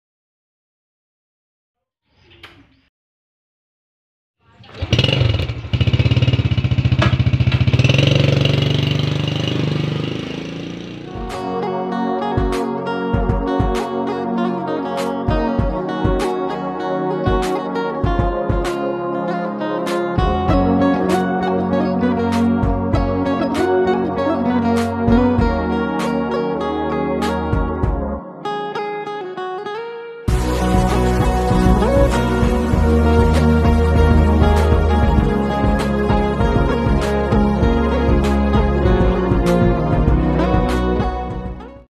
Kisah perjuangan kurir J&T Empang menghalau hujan dan panas
Kisah perjuangan kurir J&T Empang menghalau hujan dan panas demi tercapainya sebuah titipan kepada tuannya (Paket).